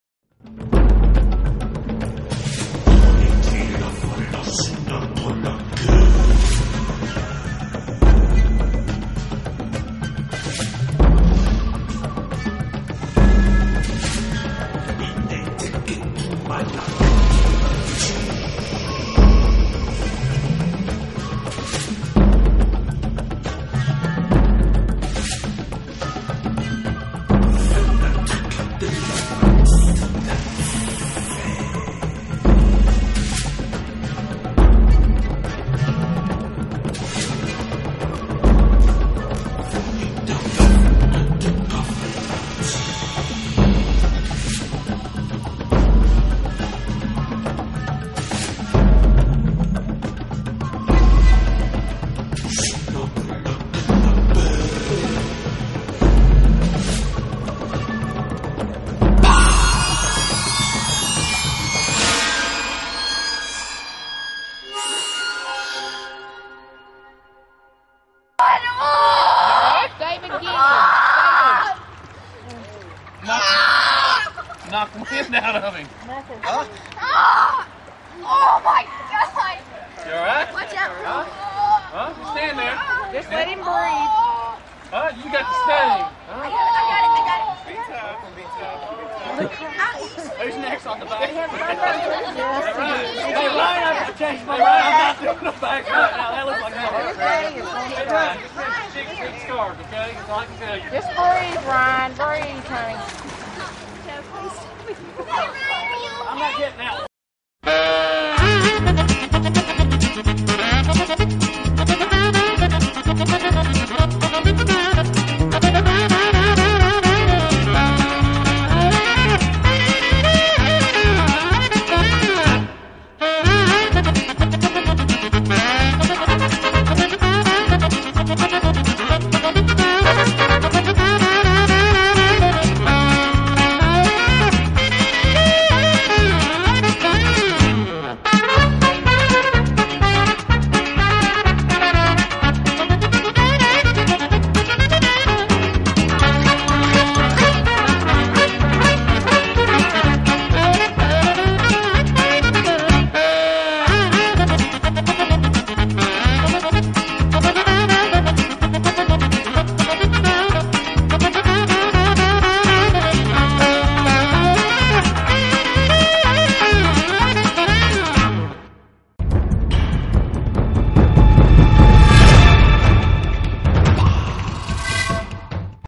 Rope swing belly buster
Ryan busts it while trying a back flip!